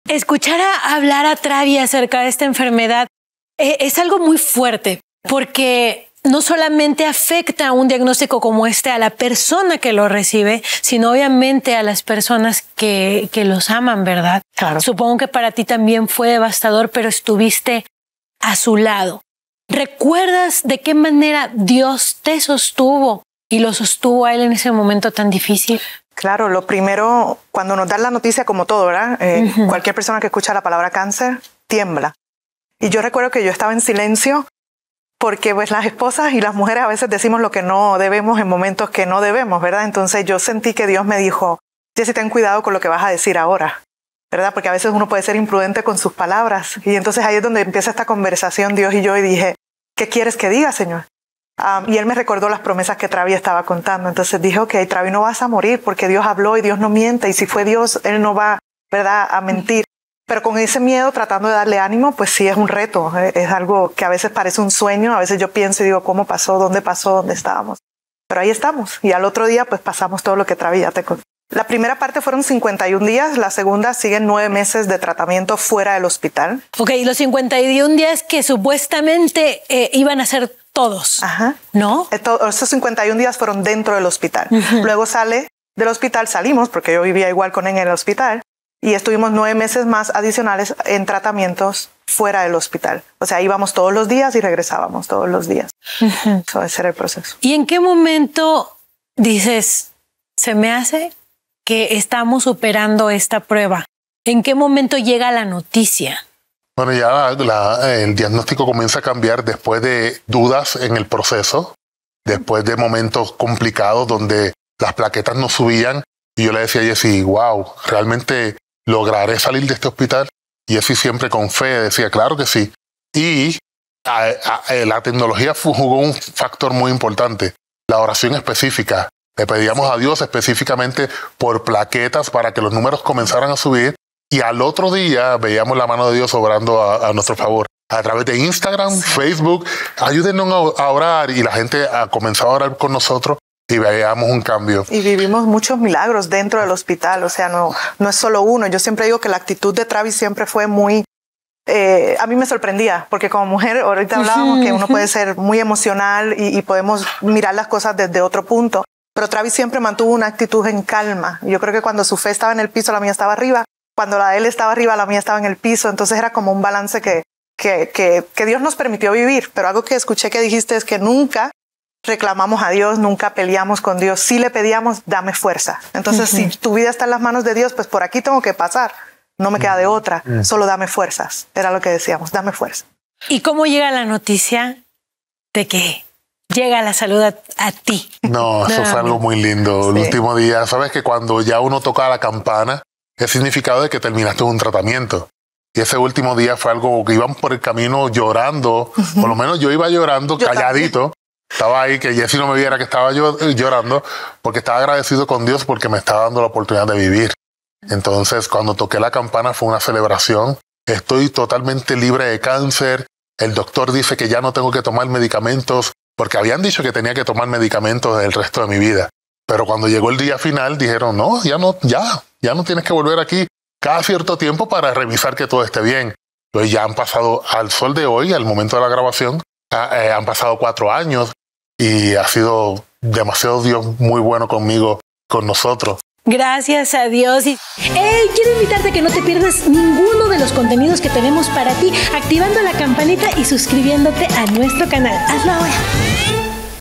Dios lo Curó del Cáncer - La historia de Travy Joe [Testimonio Real] | Vive Más Tv
En esta emocionante entrevista, @TravyJoe  comparte su increíble testimonio de fe y superación. Descubre cómo un diagnóstico devastador no pudo detenerlo y cómo experimentó un verdadero milagro de Dios. Únete a nosotros mientras exploramos su inspiradora historia y los momentos que cambiaron su vida para siempre.

¿Necesitas hablar con alguien? Mándanos un WhatsApp 🟢📲

🔔 En vivo todos los Sábados por el canal 7.2 de  Azteca + , Facebook y YouTube.